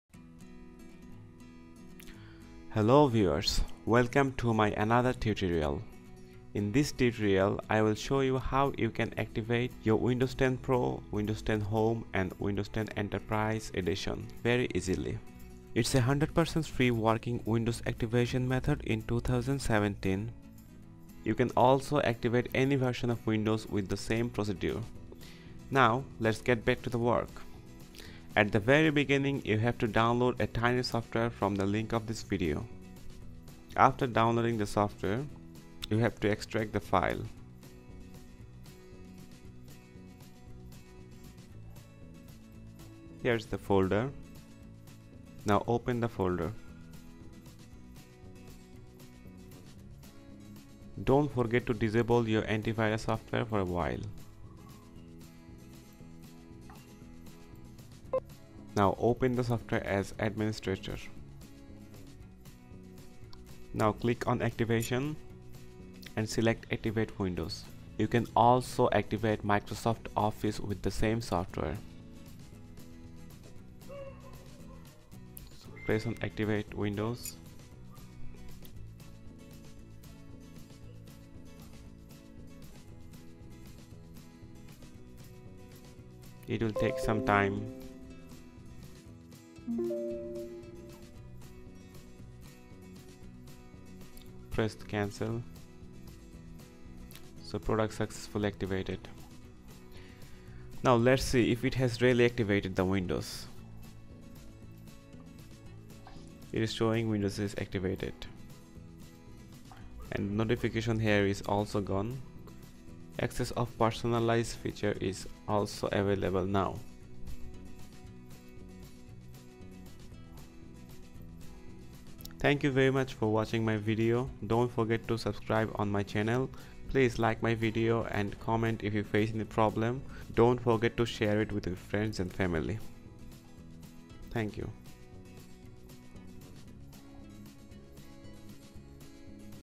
ACTIVATE ALL WINDOWS 10 VERSION IN 2 MINUTES !!
This tutorial shows you how you can activate windows 10 any version very easily and quickly.This is 100% free way to activate all the version of windows 10.The software which I have installed is a portable software that means you don't have to install the software to activate your windows.